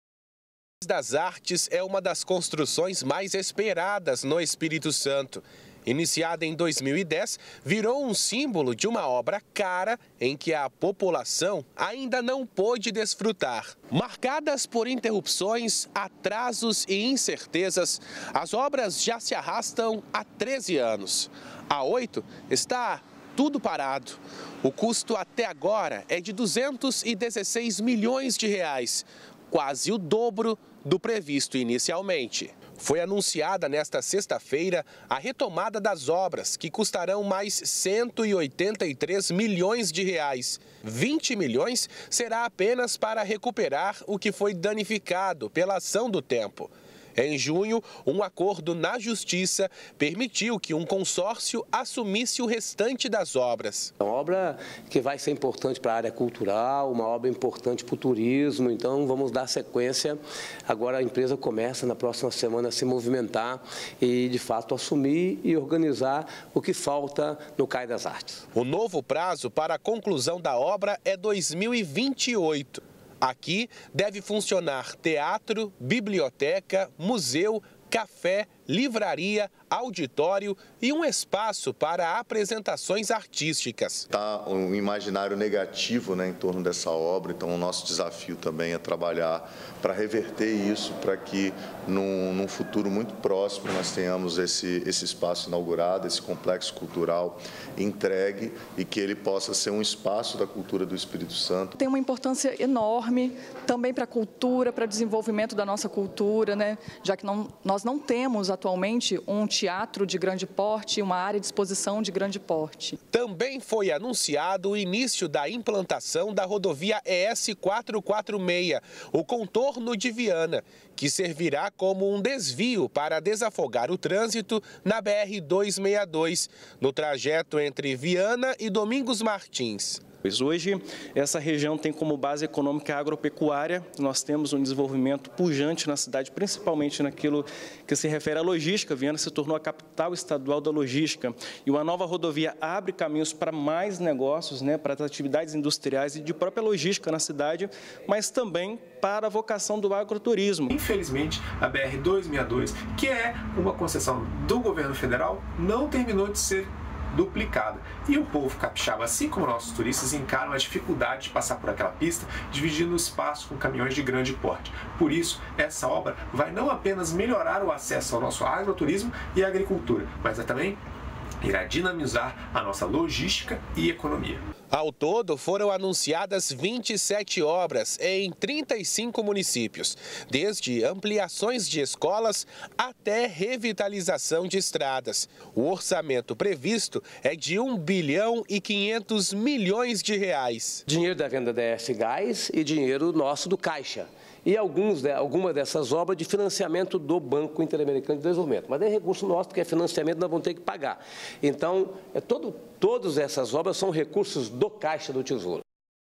Obras devem ser retomadas aqui no Estado, uma delas se tornou um elefante branco
➡️Facebook: Jornal da TV Vitória - ES